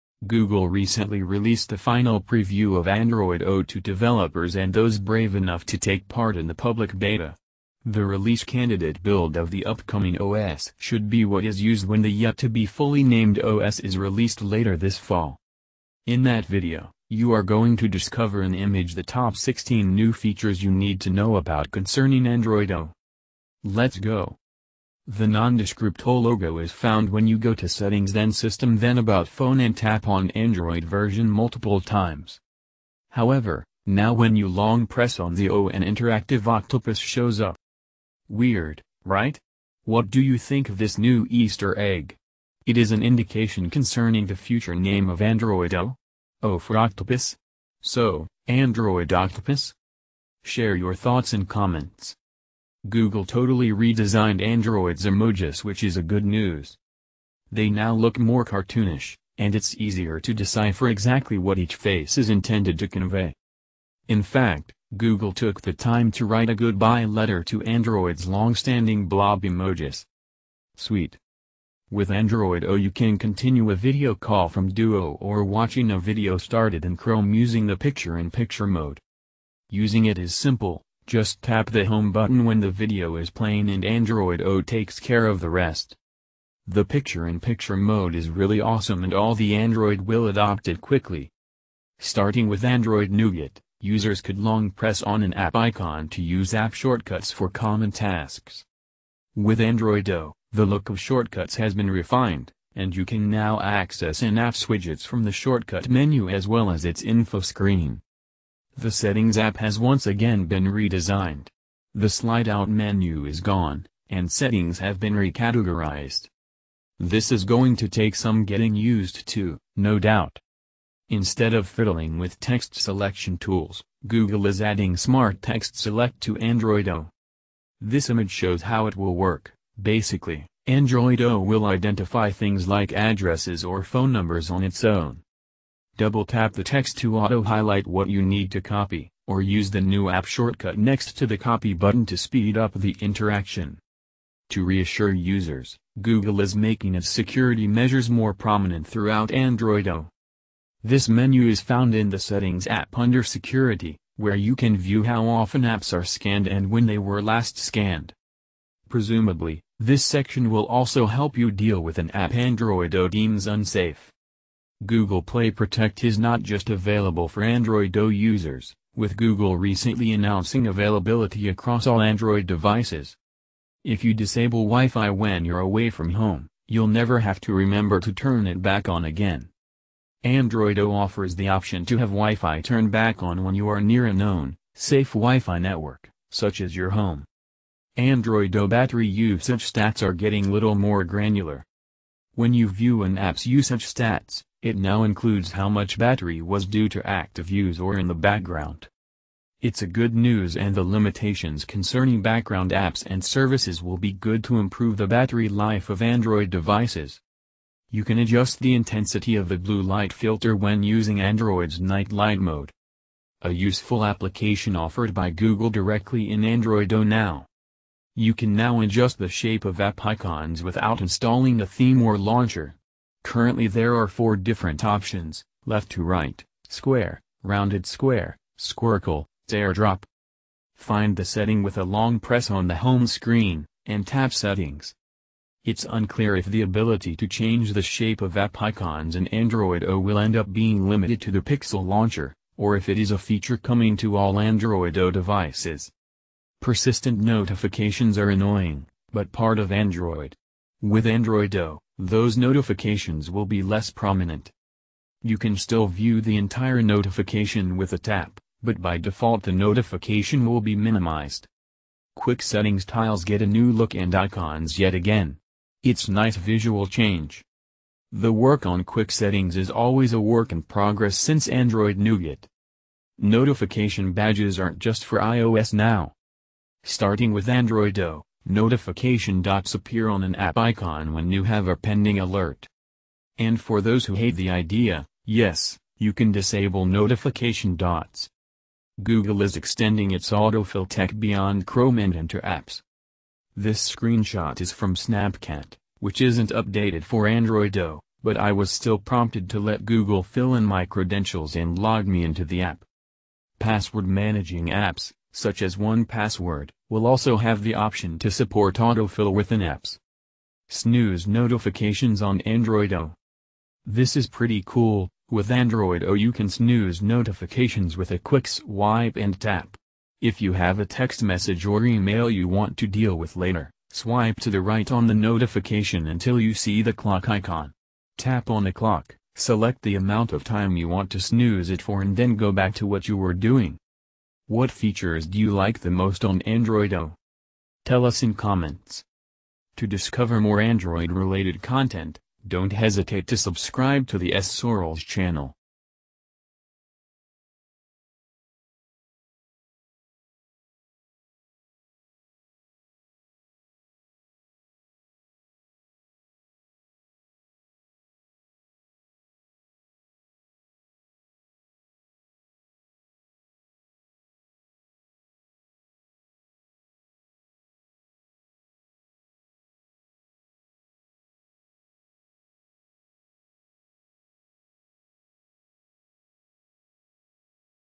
Android O : Top 16 Features you need to know about
Google has just released the final developer preview of Android to developers. Android O comes with a lot of new exciting features for users and developers. In the following video, you are going to discover the Top 16 Features you need to know about concerning Android O. Note that the final version of Android O should be released later this fall with the official name of Android O.

Let us know your thoughts concerning Android O and its future name in comments.